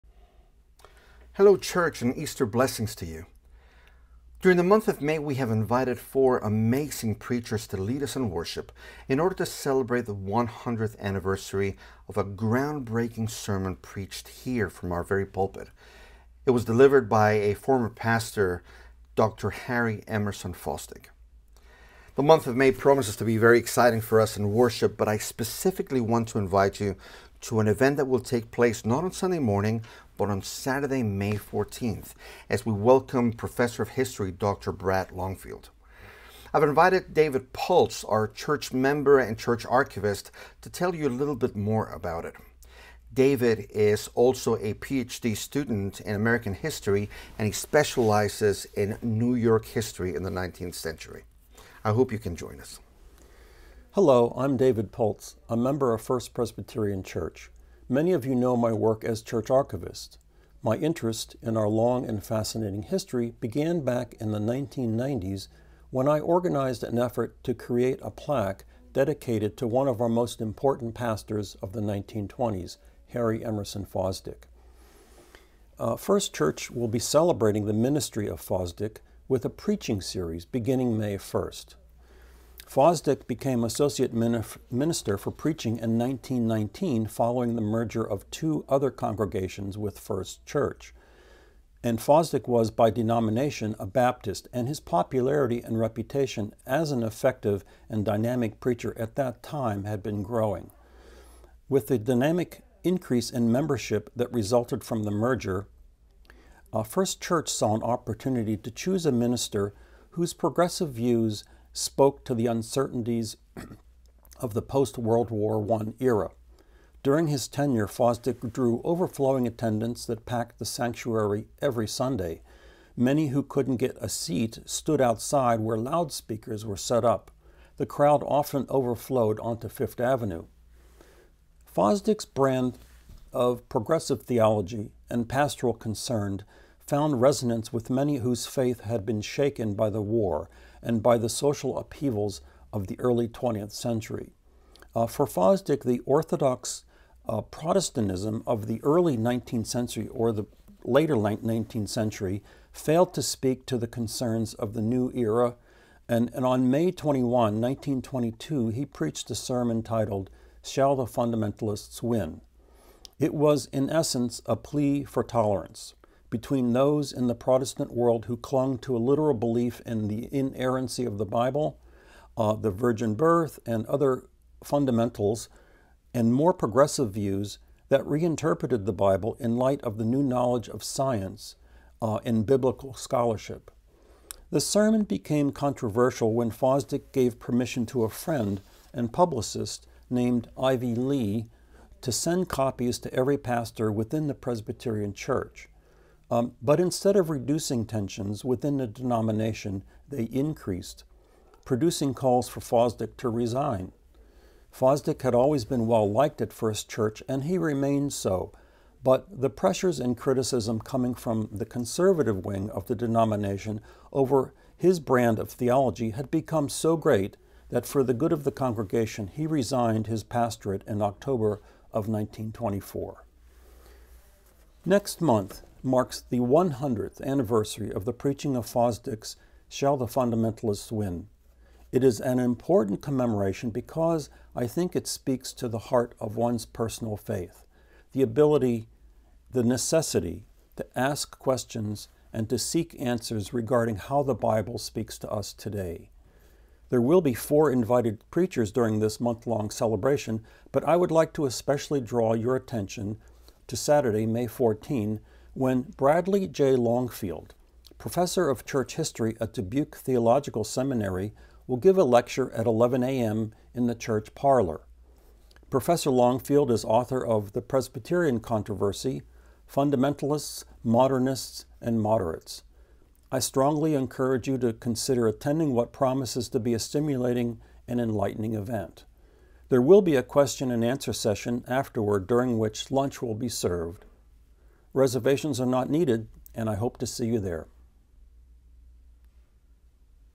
Harry Emerson Fosdick Invitation
Who was former pastor Harry Emerson Fosdick? What is Fosdick’s “Shall the Fundamentalists Win?” sermon about? What was controversial about it? Why did it cause such national controversy?  What was scandalous about it? How did a single sermon shape the ministry of First Church for years to come? What was the historical context in NYC at the time of its delivery?

Dr. Brad Longfield, professor of Church History at the University of Dubuque Theological Seminary will address these questions in a lecture on Saturday, May 14th at 11:00 a.m. in the Parlor.
A light lunch will be served during the Q&A section following the lecture. No reservation is needed.